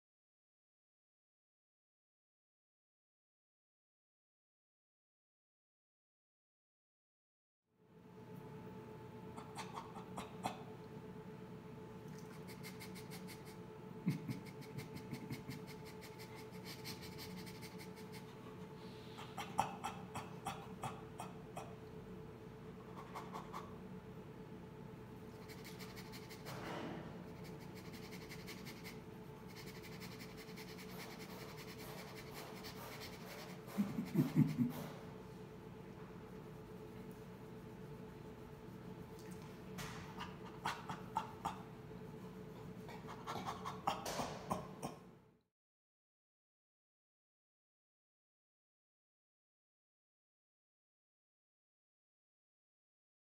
Good Morning, Neggie!
Mornings are generally a rambunctious time at the sanctuary, but if we're lucky, Burrito will hold off on his dominance displays long enough for us to say a quiet "good morning" to the group. We use the chimps' language as much as possible, which means we greet them with breathy panting, soft grunts, and head bobbing. Negra is usually the last to get out of bed, and her sleepy greetings are a perfect way to start the day.